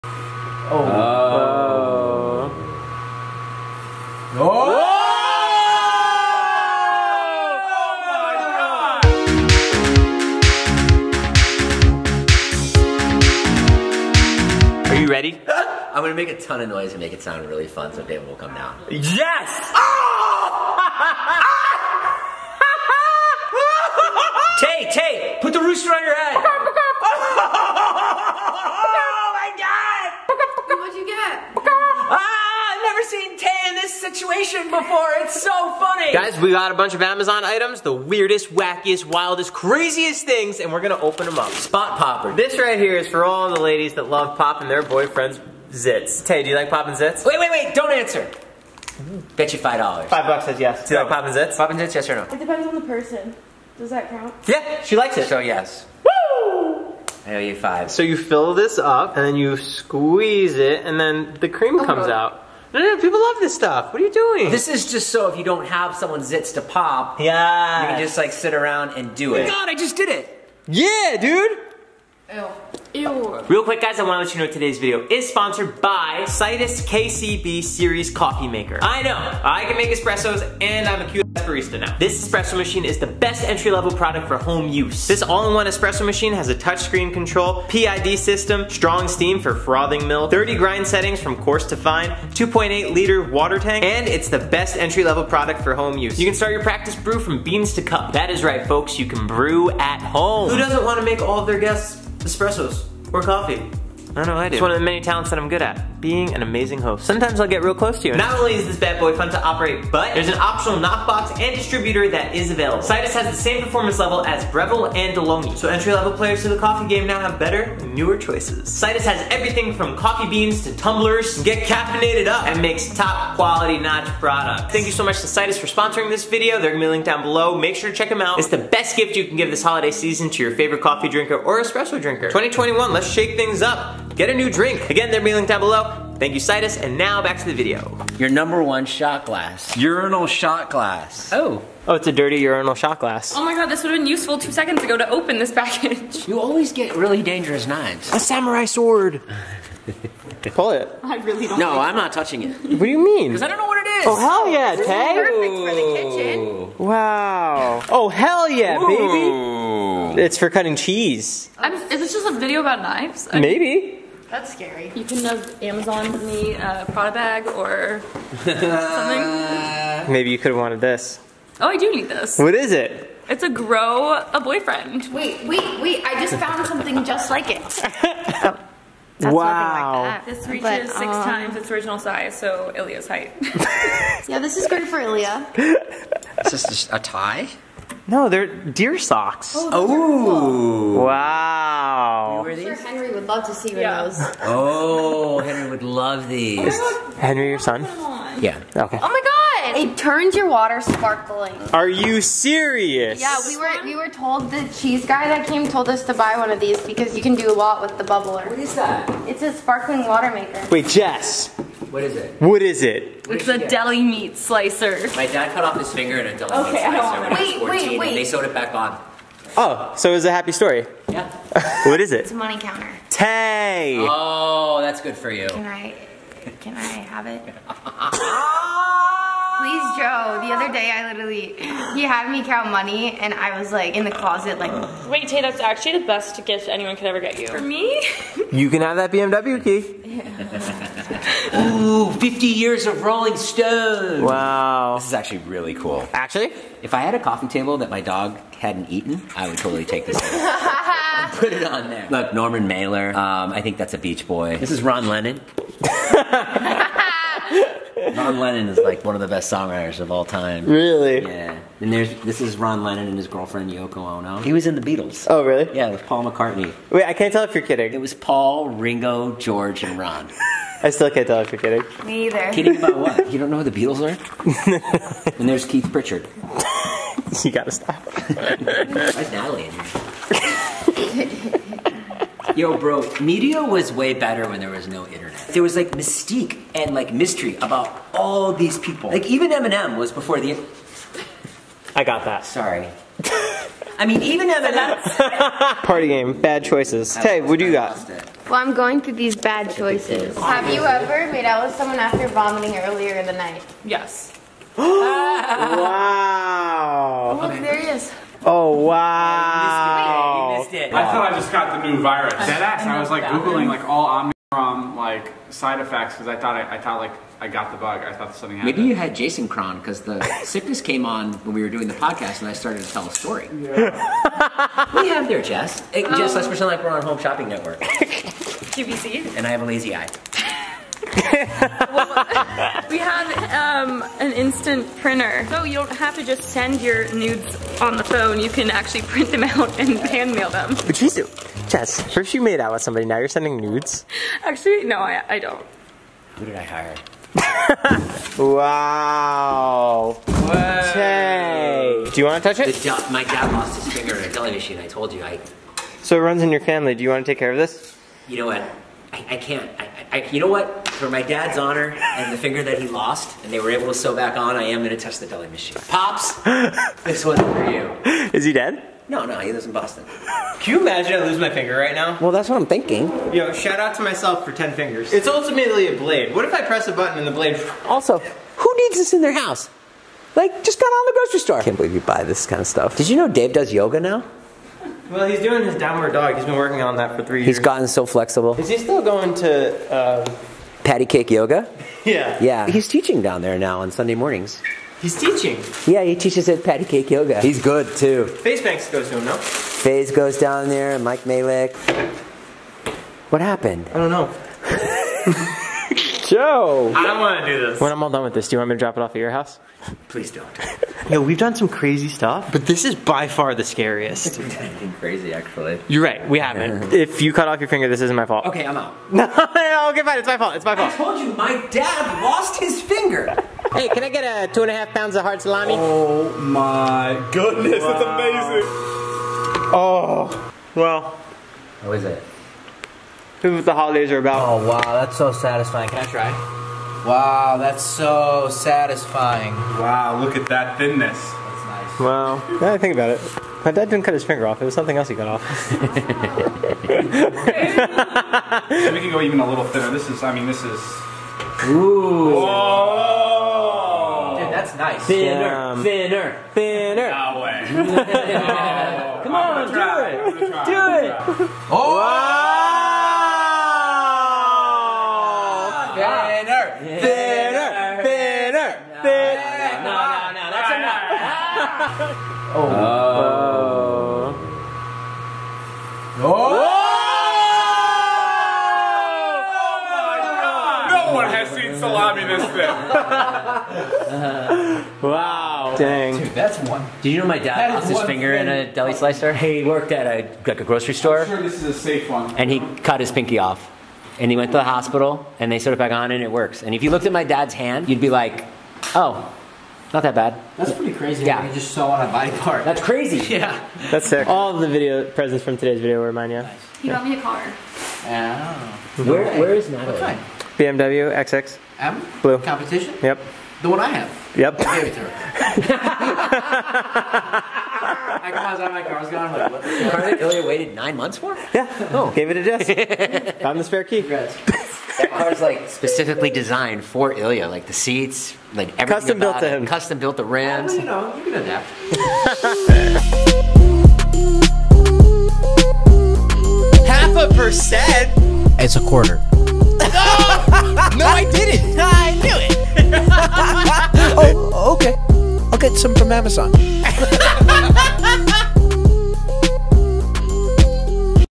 UNBOXING THE CRAZIEST AMAZON PURCHASES (2021)
We unbox the craziest most fun amazon items we could find this holiday season!!!

Thank you fellow joetatoe heads,

love,
your leader, Papa Potatoe